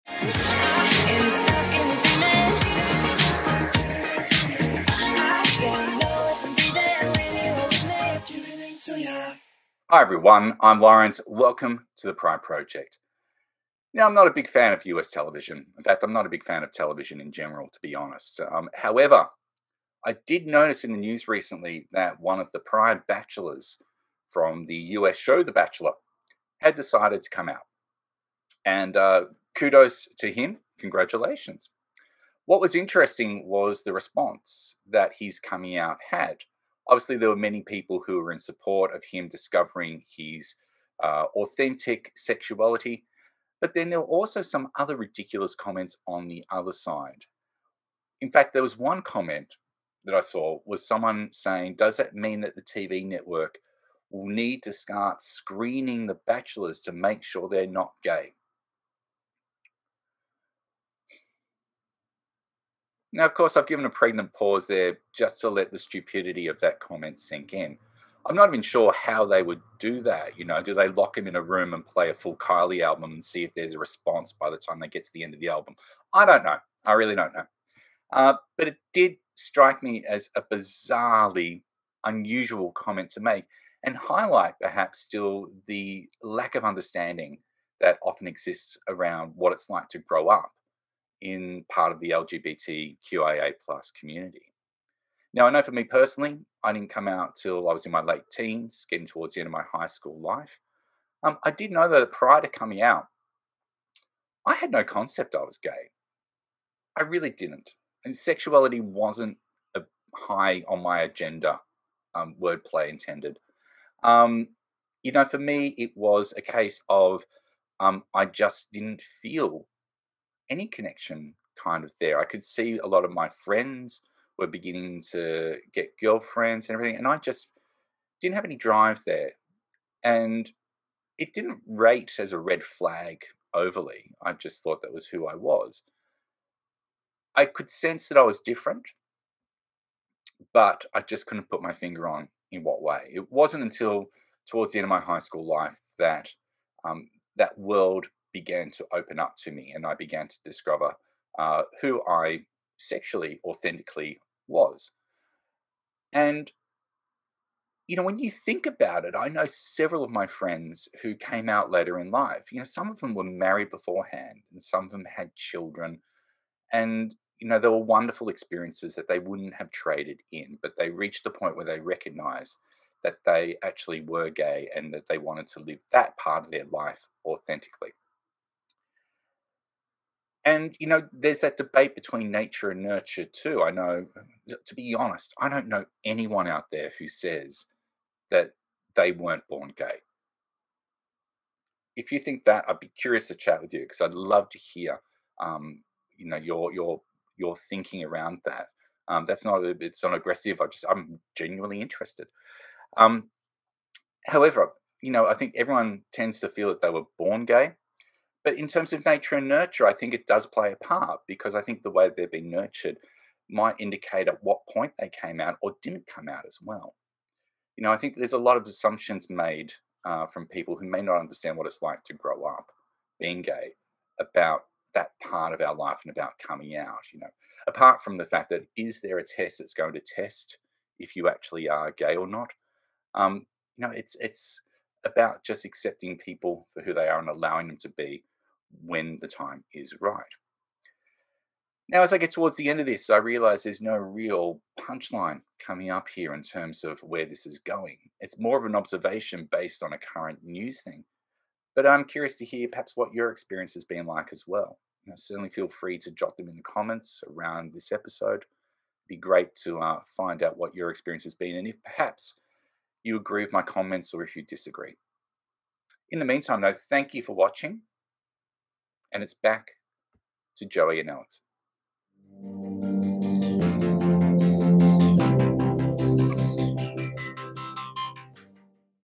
GAY SCREENING ON REALITY TV - Lawrence Akers - THE PRIDE PROJECT - SMB TV.
Two years ago, Colton Underwood was cast as the hunky star of the hyper-masculine fairytale dating show, “The Bachelor.”

Today, the reality star came out as gay, sharing his true and authentic self with the world.

In conversation with Robin Roberts on “Good Morning America,” Underwood — a former pro football player who grew up in a conservative, Catholic community — reflected on how the show impacted his identity with his sexuality.

“I literally remember praying to God the morning I found out that I was ‘The Bachelor’ and thanking Him for making me straight,” Underwood said. “I remember that vividly, of saying, ‘Finally, you’re letting me be straight. Finally, you’re giving me a wife, a fiancée, and then I’m going to have the kids, then I’m going to have the house, and then I’m going to have all this.’”

When Underwood starred on “The Bachelor” in 2019, he was heavily promoted as “The Virgin Bachelor” — something that he later told Variety he believed was “overblown,” but not necessarily exploited.

On Wednesday morning, speaking his full truth, Underwood explained, “I could never give anybody a good enough answer about why I was a virgin. The truth is I was the ‘Virgin Bachelor’ because I was gay, and I didn’t know how to handle it.”

In support of Underwood coming out, the executive producers of “The Bachelor” franchise released a statement on Wednesday.

“We are so inspired by Colton Underwood’s courage to embrace and pursue his authentic self,” the statement reads. “As firm believers in the power of love, we celebrate Colton’s journey in the LGBTQIA+ community every step of the way.”

The Secret MENS Business podcast:
Google Manager
Amazon Music
Podomatic -

secretmensbusiness_smb